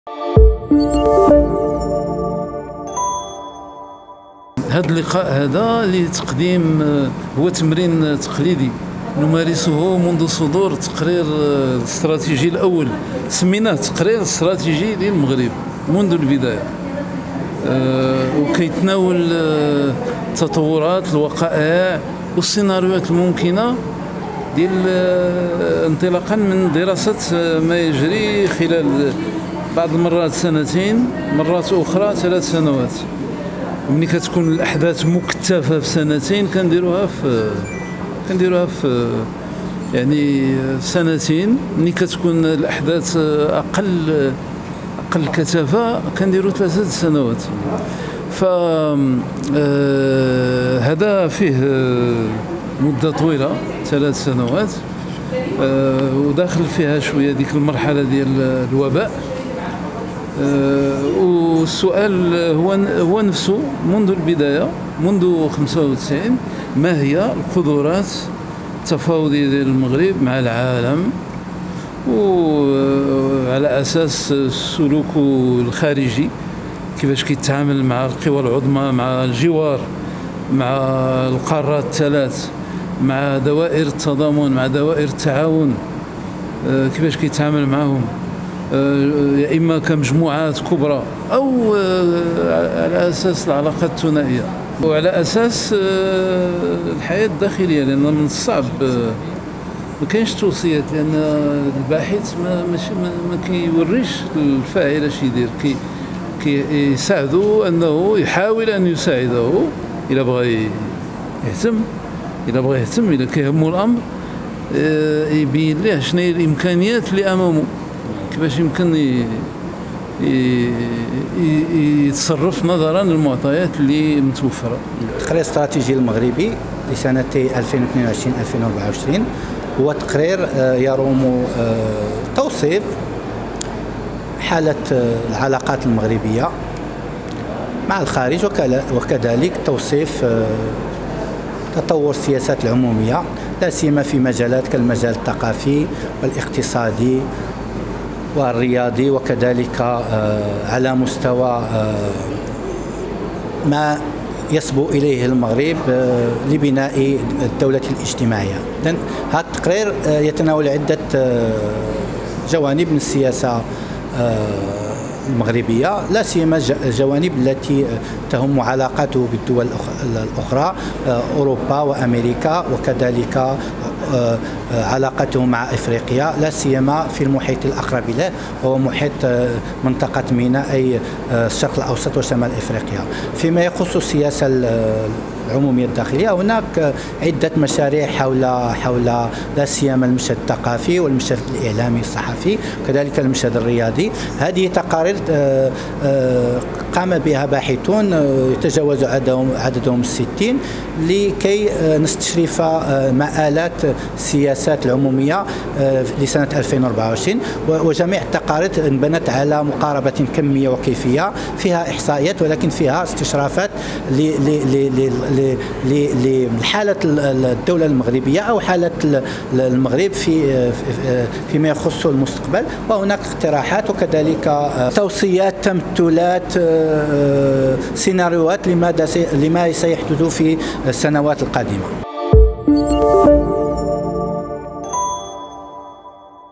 مركز دراسات يقدم "التقرير الاستراتيجي المغربي للفترة 2020-2024 "
نظم "مركز الدراسات والأبحاث في العلوم الاجتماعية" CERSS الذي يرأسه عبد الله ساعف، اليوم الجمعة 12 يوليوز 2024 لقاء أكاديميا من أجل تقديم الإصدار الجديد الخاص بـ "التقرير الإستراتيجي المغربي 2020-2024".